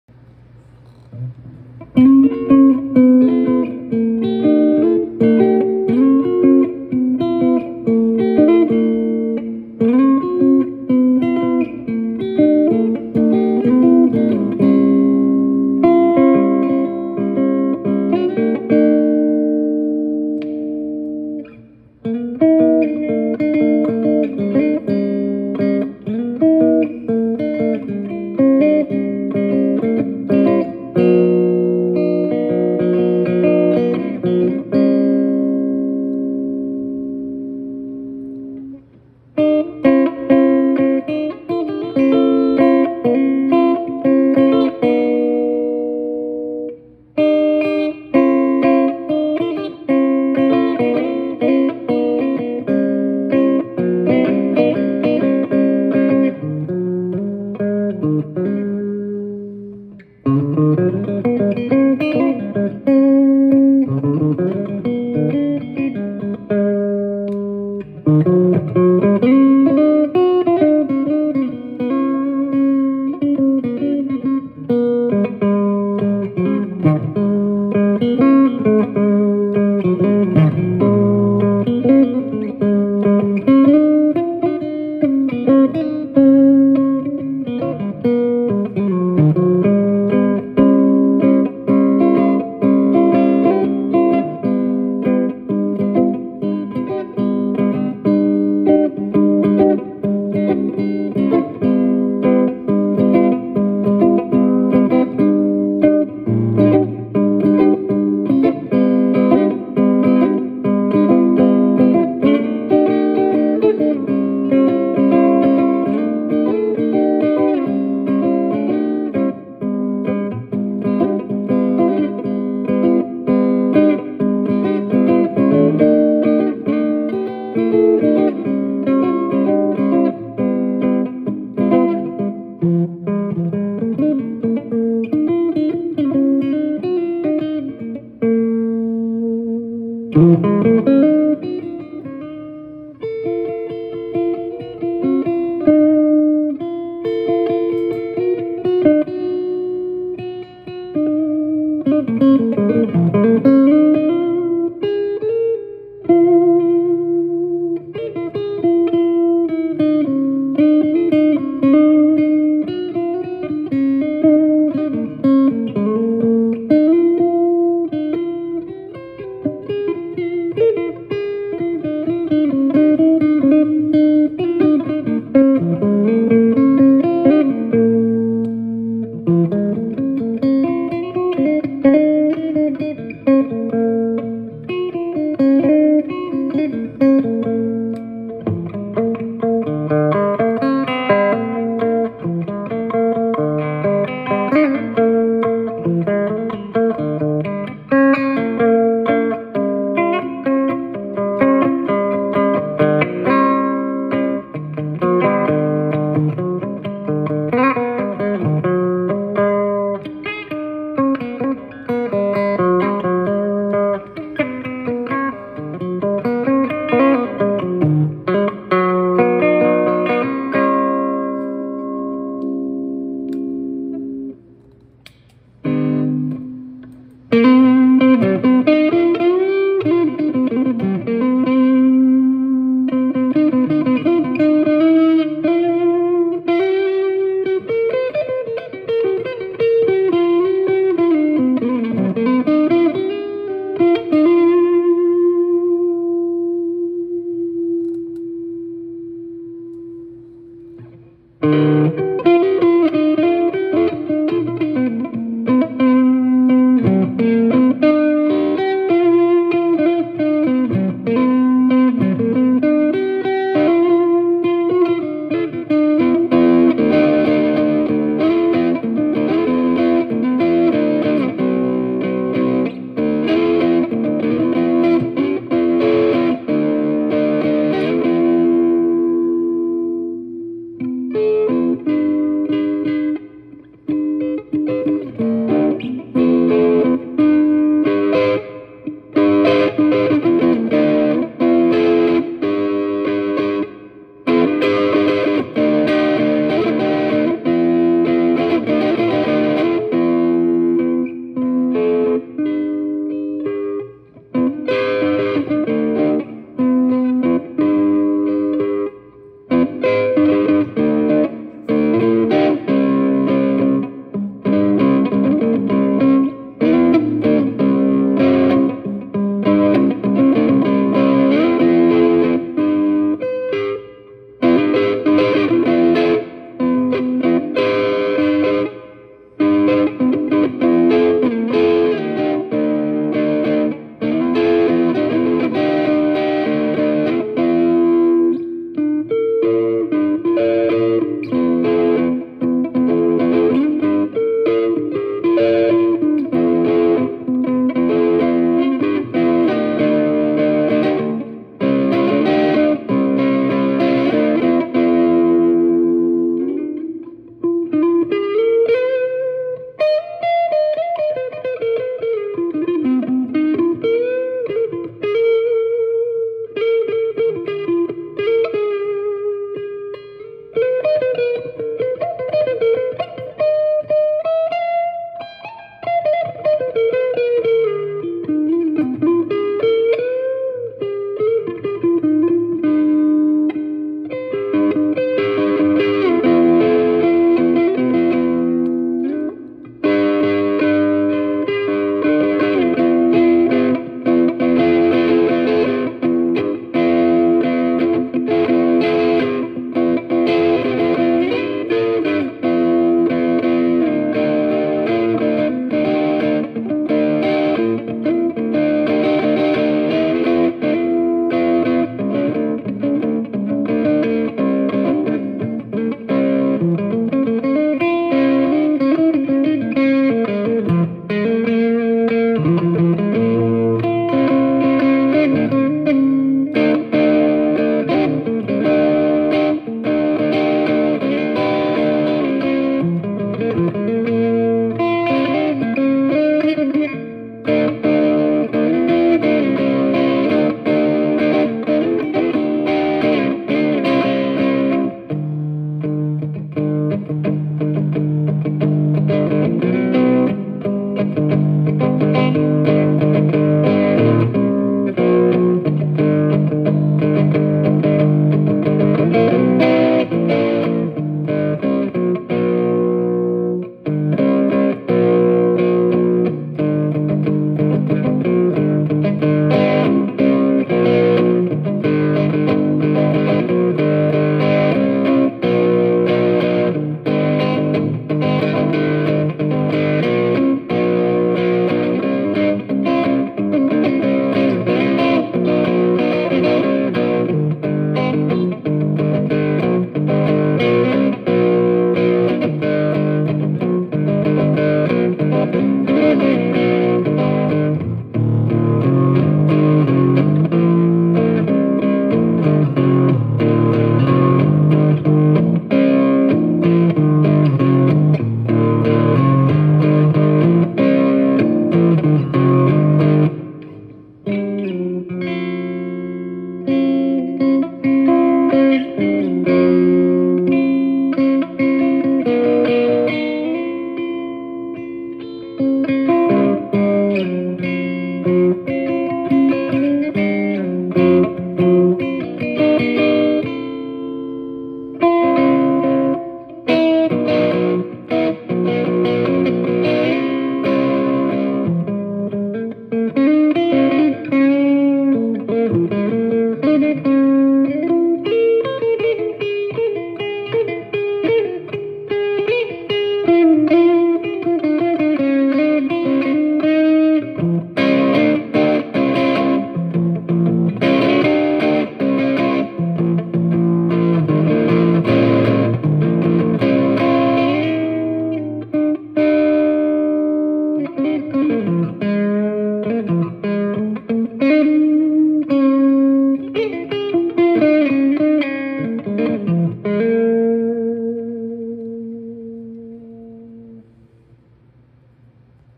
2019 Les Paul Tribute Honey Burst w Donner Dumbal Pedal - Clean & Dirty
Loving the warm, full pickups on this Honey Burst ! And with the added drive from the transparent Donner Dumble overdrive pedal I get rich colorful tones with power and clarity.